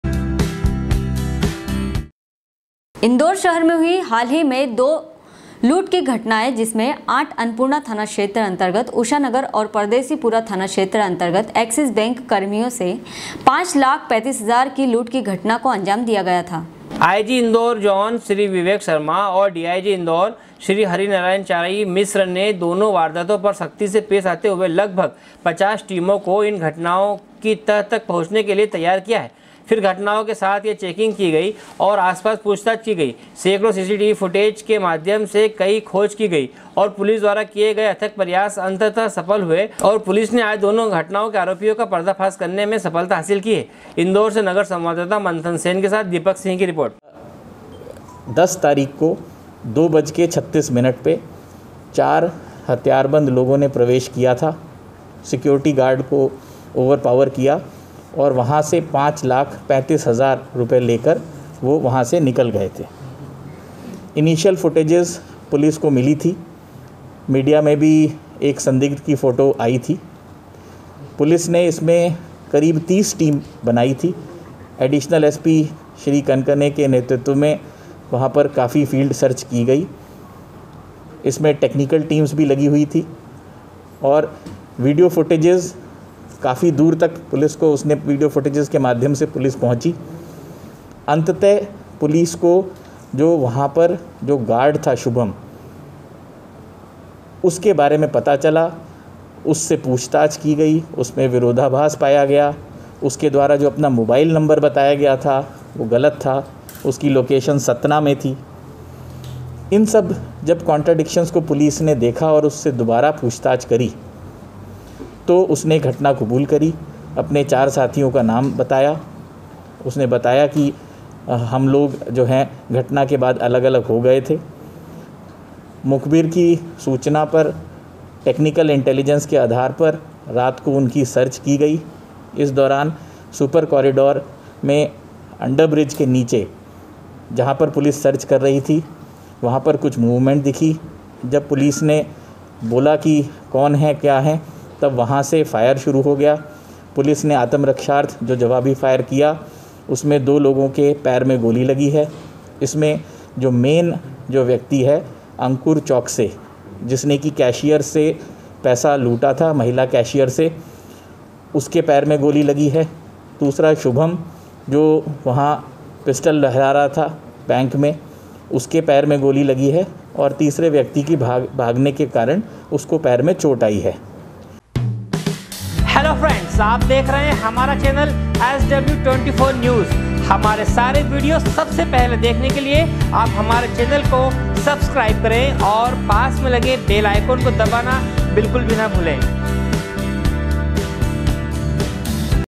SW 24 News Madhyapradesh#indore#12.07.2020
sw sw24news sw24newsmadhyapradesh livenews news24इंदौर शहर में हुई हाल ही कि दो लूट कि घटनाएं जिसमें 8 अन्नपूर्णा थाना क्षेत्र अंतर्गत उषा नगर और परदेशीपुरा थाना क्षेत्र अंतर्गत एक्सिस बैंक कर्मियों से 5.35 लाख की लूट की घटना को अंजाम दिया था।
इंदौर से नगर संवाददाता मंथन सेन के साथ दीपक सिंह की रिर्पोट।